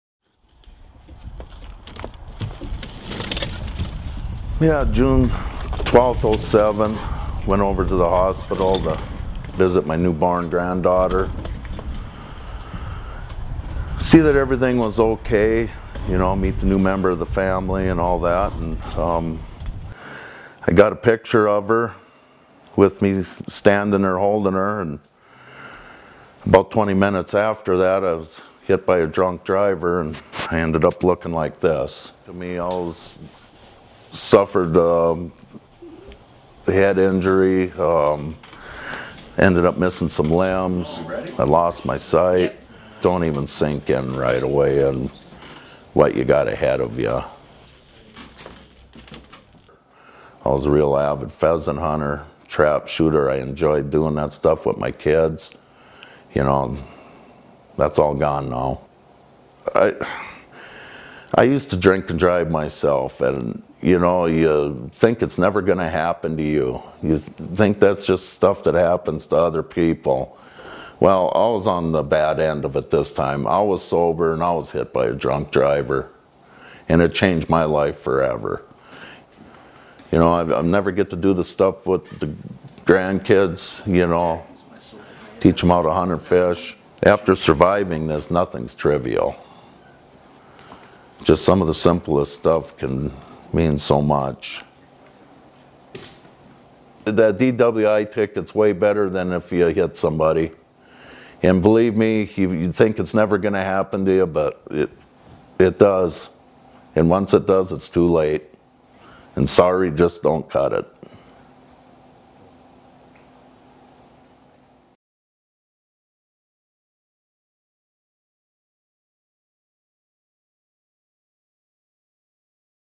Greg Torell - double amputee - 1
Greg Torell lost his left arm and leg in an accident.